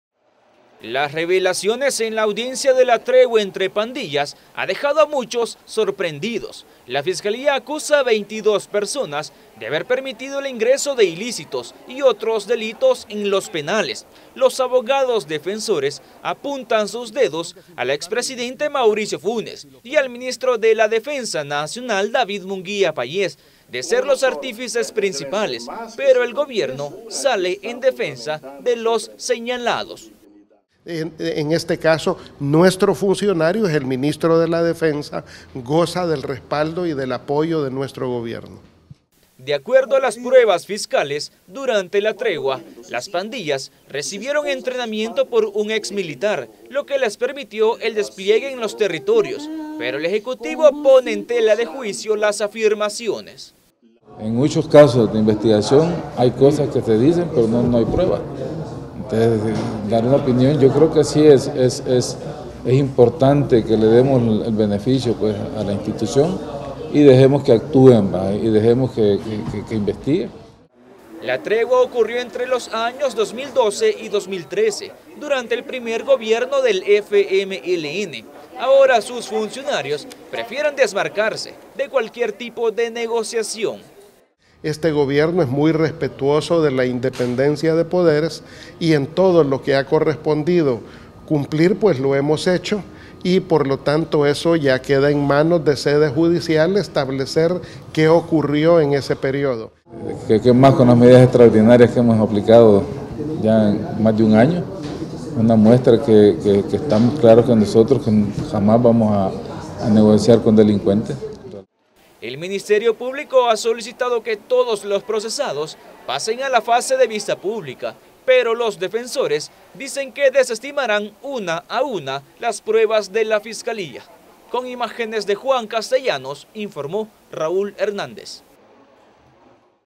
CN: Funcionarios reaccionan a caso tregua
Funcionarios de gobierno han reaccionado a las revelaciones  durante la audiencia preliminar en el caso de la tregua entre pandillas.